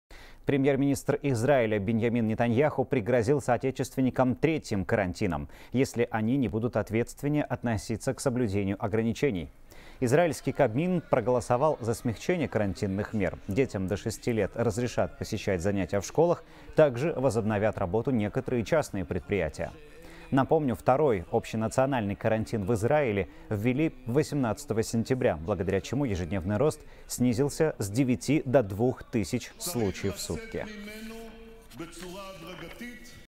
Израильский кабмин проголосовал за ослабление ограниченй
Премьер-министр Израиля Биньямин Нетаньяху пригрозил соотечественникам третьим карантином, если они не будут ответственнее относиться к соблюдению ограничений, передает «Хабар 24».